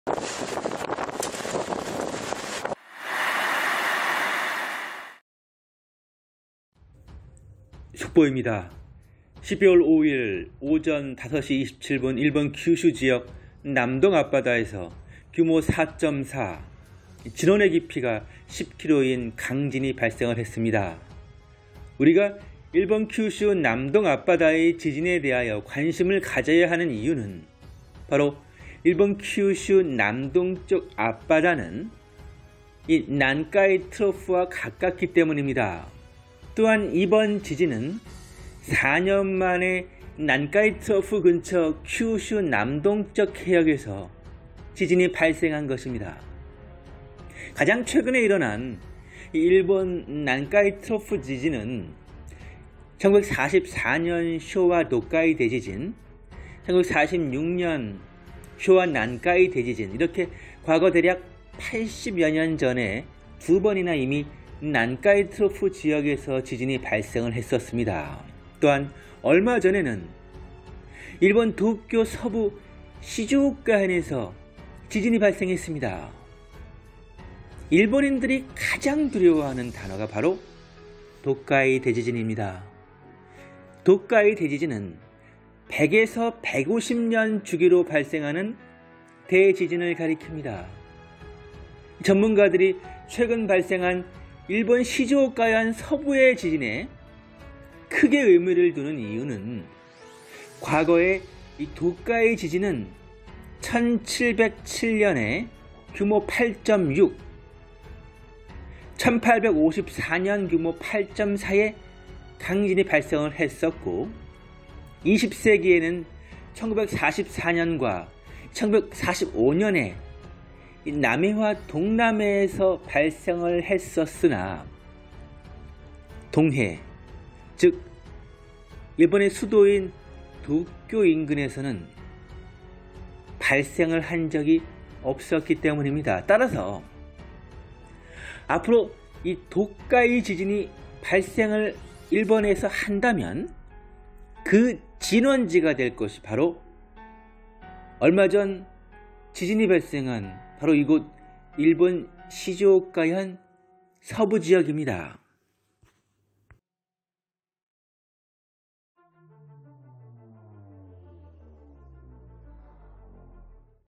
난카이 트로프에 지진 발생,도쿄,시즈오카,후지산 대폭발위기 최대 규모 9.1 발생과 34m 쓰나미 예상으로 난리 났다
12월5일 오전 5시27분 일본 규슈 지역 남동앞바다에서 규모 4.4,진원의 깊이가 10km인 강진이 발생을 하였습니다.
우리가 규슈 남동앞바다의 지진에 대하여 관심을 가져야 하는 이유는 바로 규슈 남동쪽 앞바다는 난카이 트로프와 가깝기 때문입니다.
또한, 이번 지진은 4년만에 난카이 트로프 근처 규슈 남동쪽 해역에서 지진이 발생을 한 것입니다.